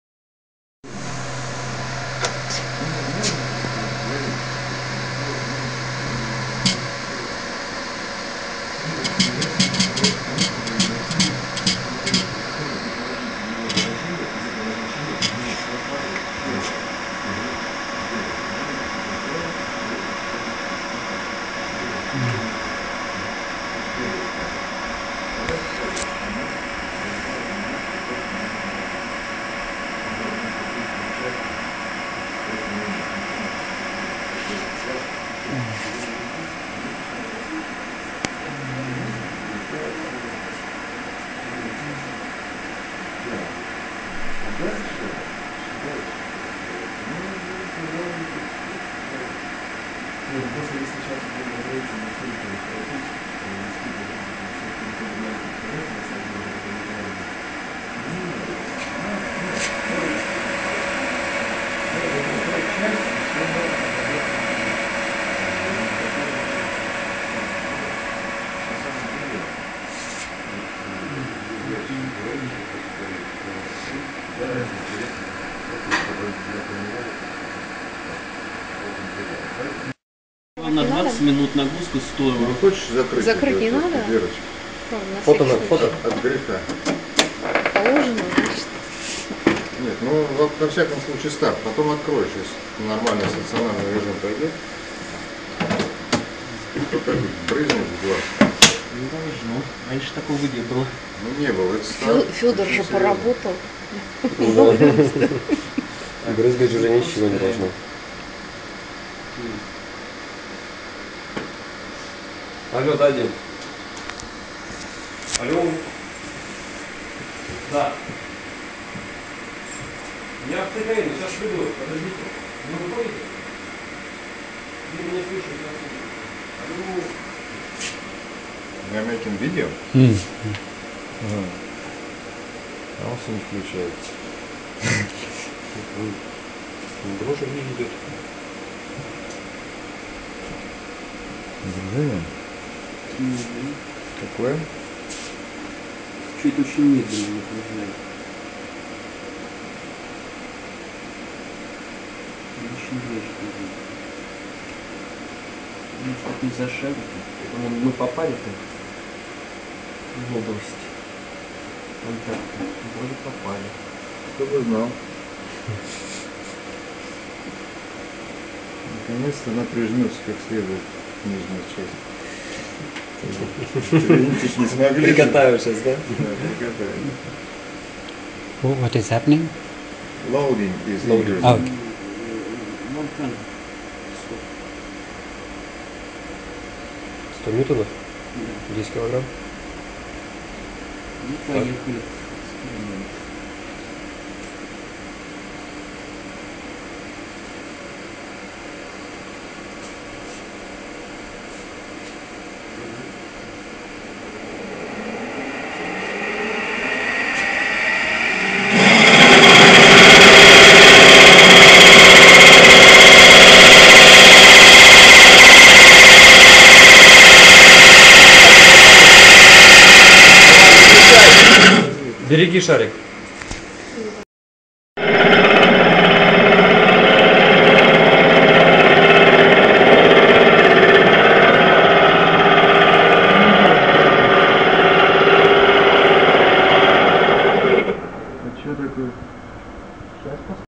Pin on disc abrasive wear test
A dry abrasion test in which a 52100 bearing steel ball with a Rockwell hardness of 62 is held stationary under load, against a rotating disc of the test material, in this case a strong steel transformed into pearlite. The experiment was recorded at MiSIS, the Moscow Iron and Steel Research Institute, NUST, Russian Federation, courtesy of Sergey Kaloshkin and Olga Ushkanova. The sample was prepared by Subhankar das Bakshi of Cambridge University